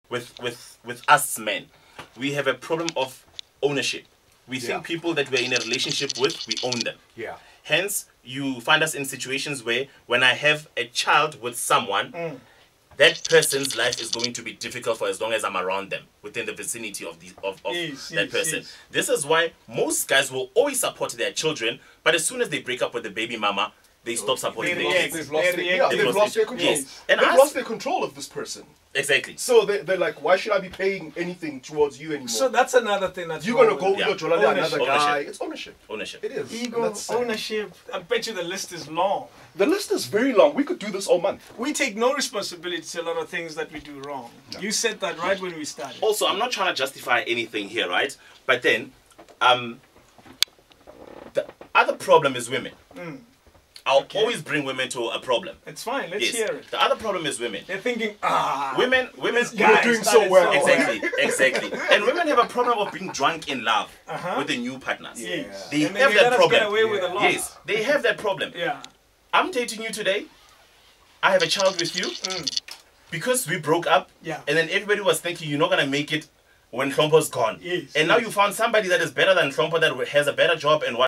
MenTalk on KingsandQueens with King DavidMashabela Hlompho Maseko and Linda Ande Yende
MenTalk on KingsandQueens with King DavidMashabela Hlompho Maseko ( @WellBehaved_Guy ) and Linda Ande Yende